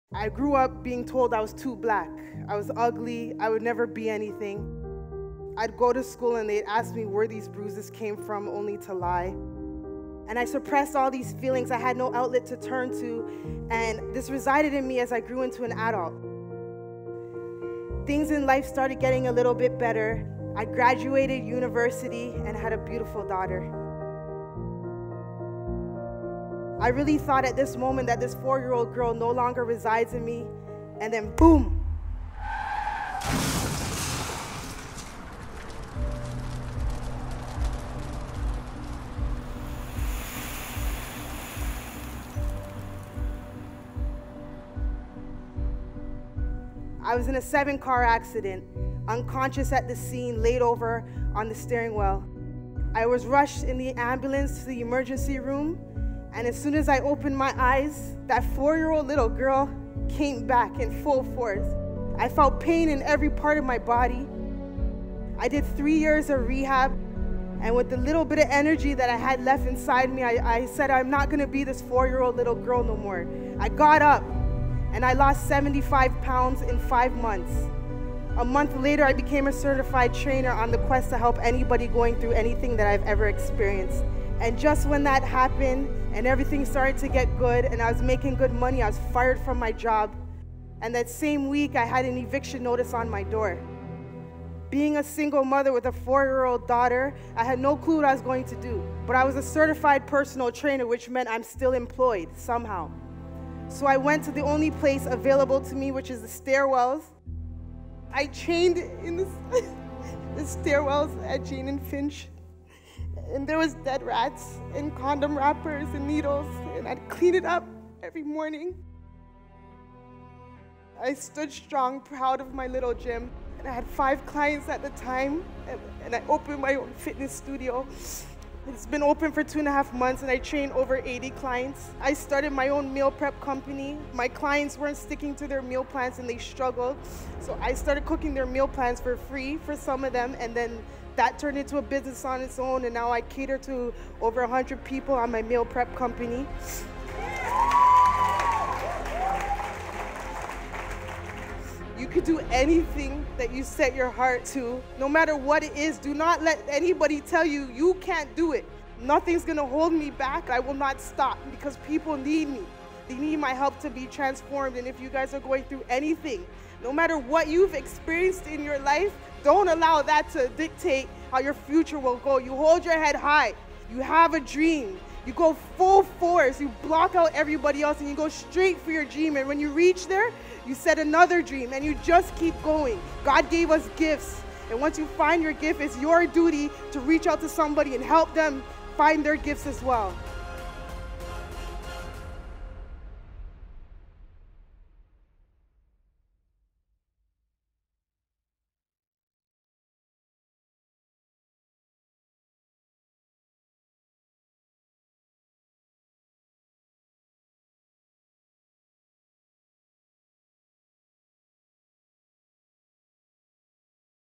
Mother Overcomes Trauma for Daughter | Shontelle Dubois | Goalcast
Shontelle's powerful story about staring down her fears and overcoming every obstacle in her way will inspire you to always hold your head high, believe in something, and go full force.